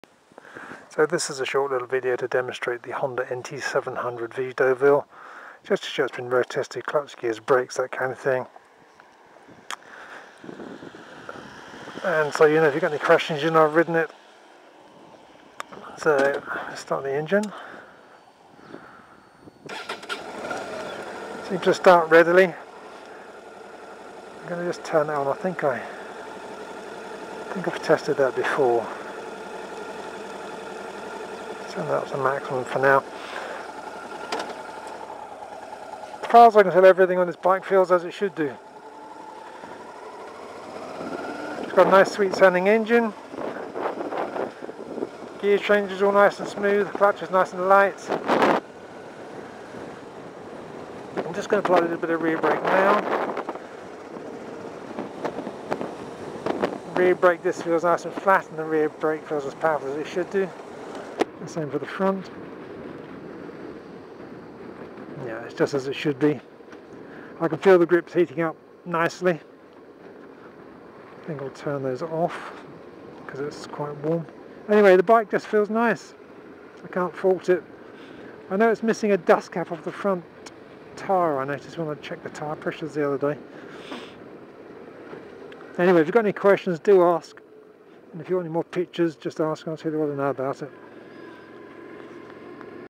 Honda NT700VA6 Deauville 60K (low cost delivery options:¬)
This Honda NT700 Deauville has come up for sale here in West Sussex.  As far as I can tell this is an all round good bike with no work to be done.

If you are interested in this bike then do get in contact and ask as many questions as you like.

Instructions to purchase.
Check with me that bike is still available.
Send £100.00 deposit via card (over the phone) or PayPal PayPal user id available on request).